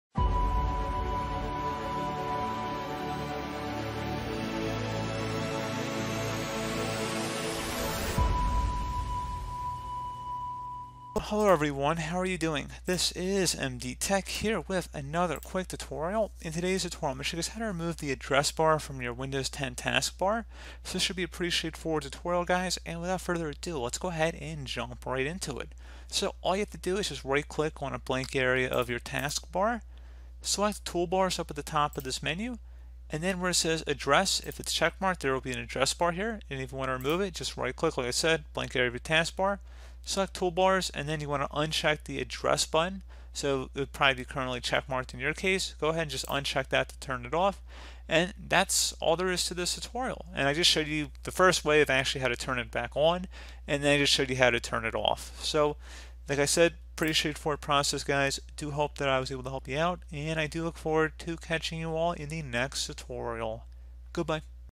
How to Remove Address Bar From Windows 10 Taskbar [Tutorial]
We’ve seen Windows 10 evolve with each updated build that has been released and a very prominent, much talked about feature that was introduced was the new search bar added next to the start button. It takes up space on the Taskbar which we usually reserve for our frequently used apps and it is somewhat redundant since you can still search your computer from the Start Menu. Fortunately, there are two very simple ways to disable the Search bar so that it doesn’t appear on the Taskbar in Windows 10.

Issues addressed in this tutorial:
taskbar in windows 10